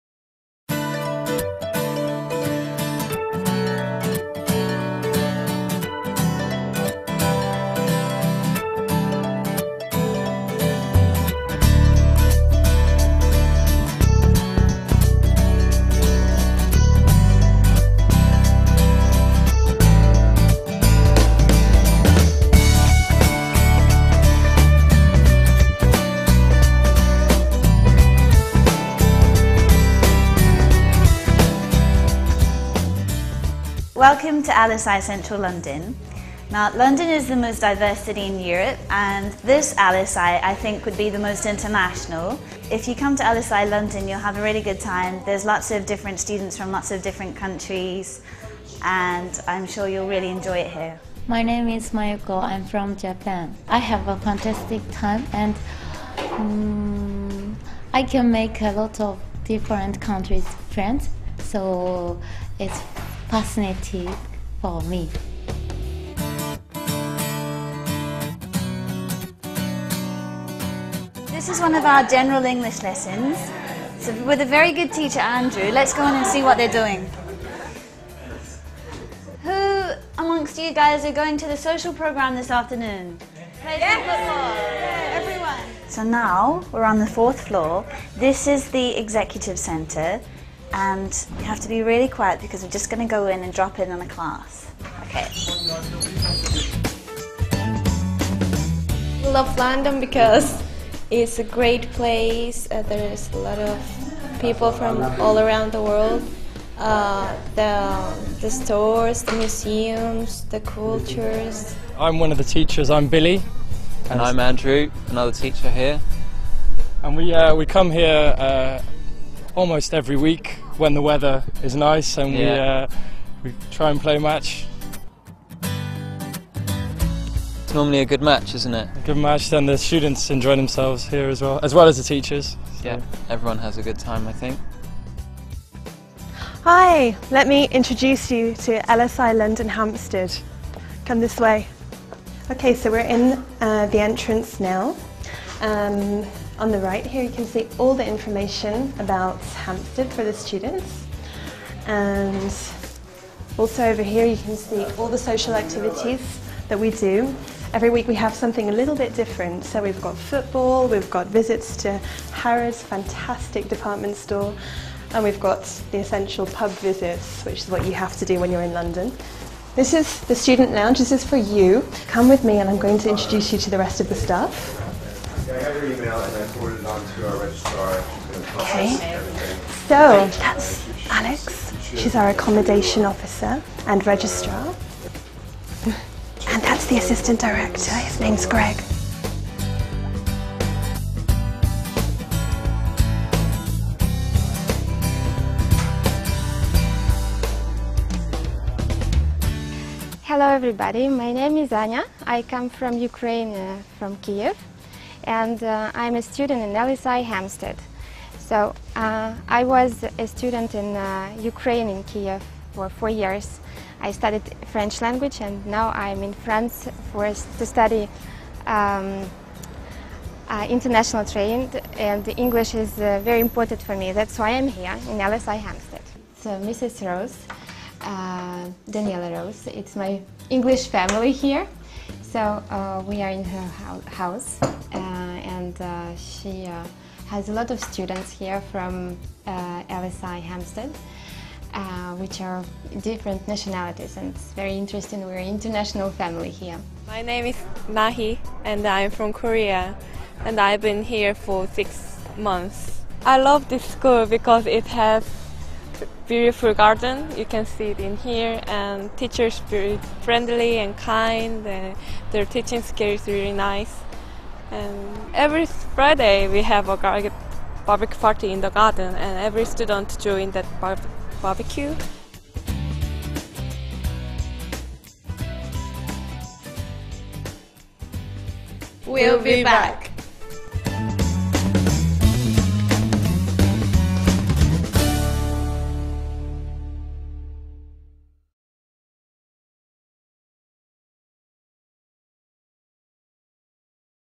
Языковая школа Language School London Central (LSI)
Языковая школа Language School London Central (LSI) является структурным подразделением международной образовательной сети Language Studies International.

С момента основания (1965 год) школа в Лондоне пользуется огромной популярностью у студентов со всего мира, благодаря качественному обучению и прекрасной учебной инфраструктуре.

Ежегодно в языковых школах сети Language Studies International проходят обучение тысячи студентов со всех уголков мира.